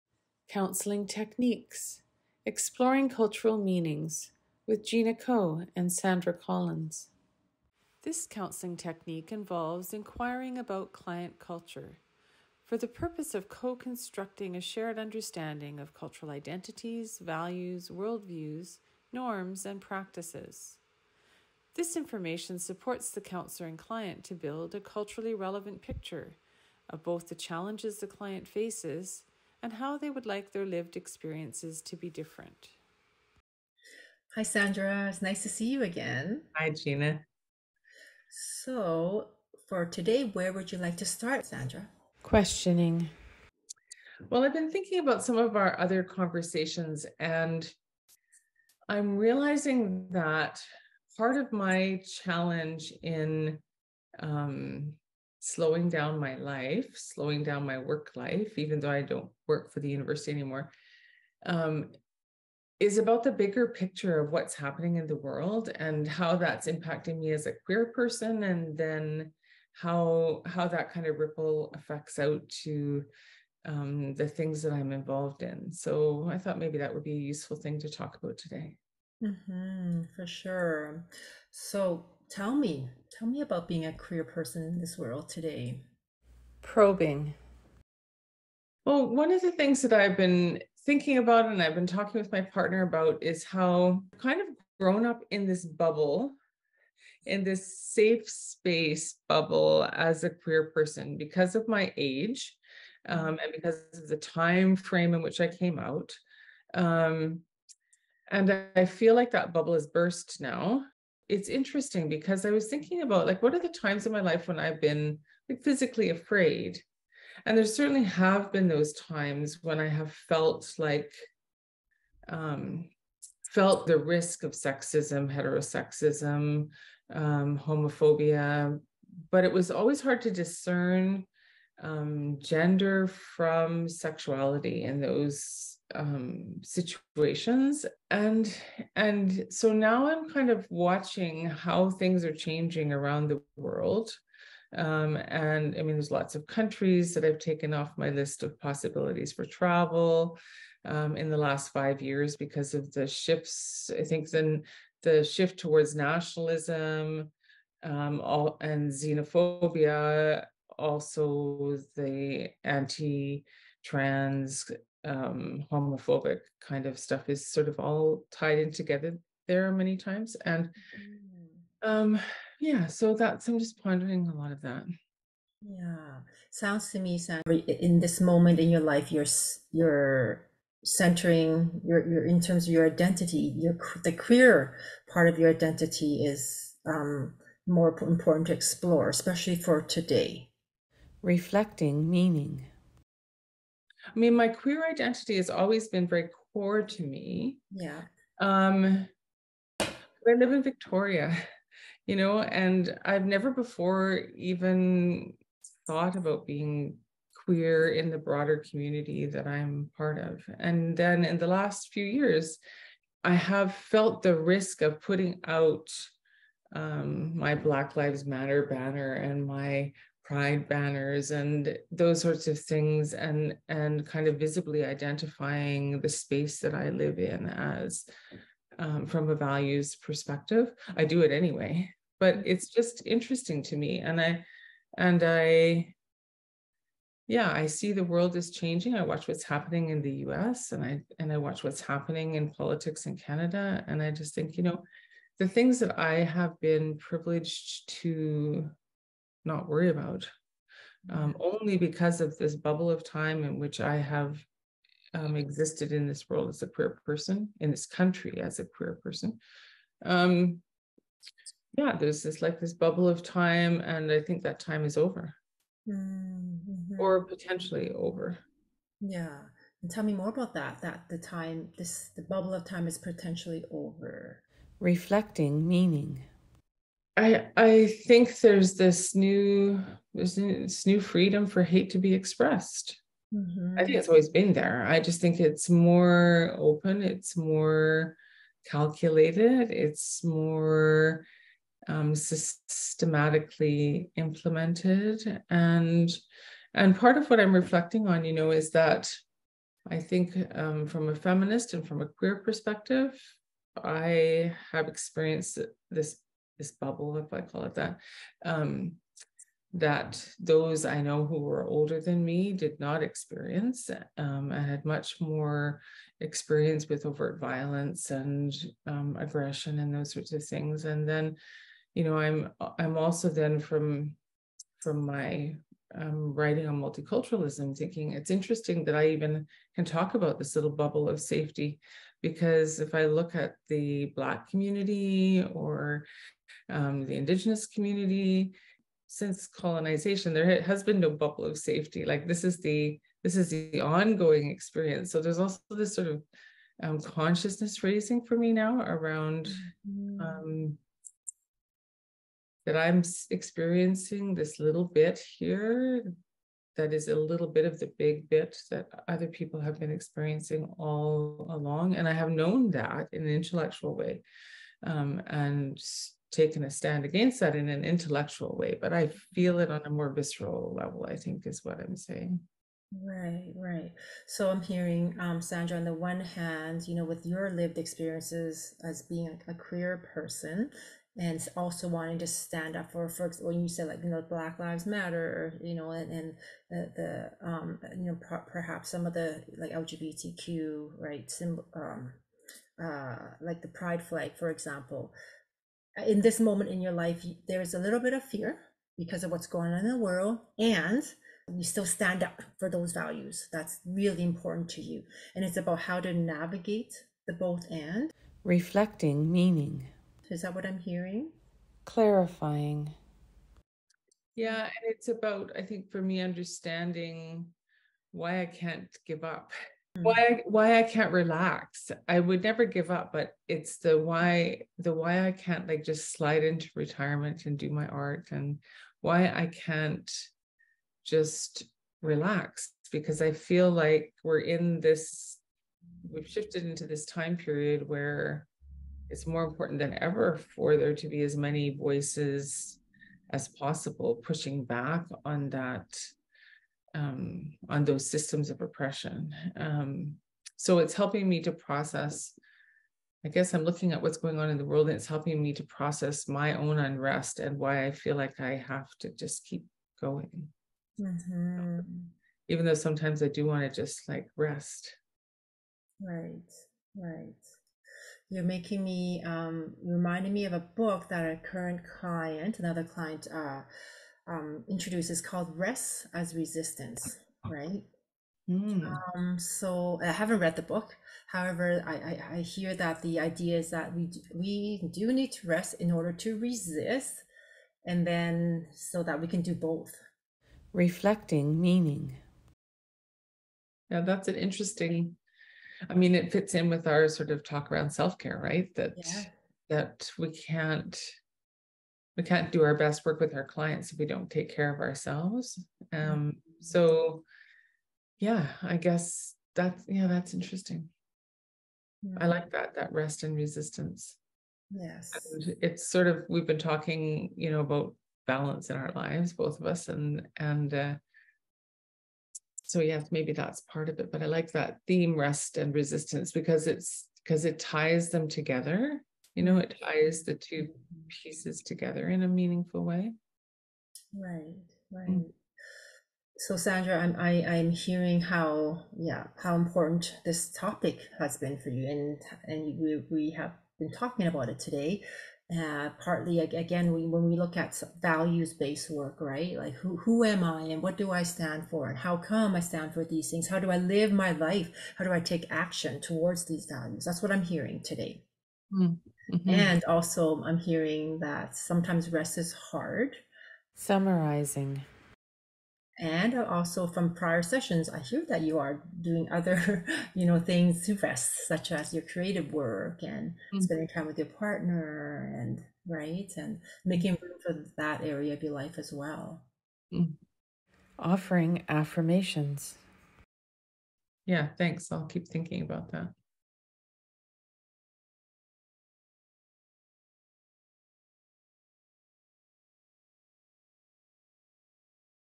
Counselling Techniques: Exploring Cultural Meanings II
Counsellors use the technique of exploring cultural meanings to inquire about client culture so that you can co-construct a shared understanding of their cultural identities, values, worldview, norms, and practices. The purpose of this technique is to demonstrate cultural humility on the part of the counsellor, to assess the client's cultural identities and social location, and to actively explore client values, beliefs, and worldview.

You can view descriptions and video demonstrations for the microskills used in this video and other techniques through my Counselling Microskills and Techniques playlist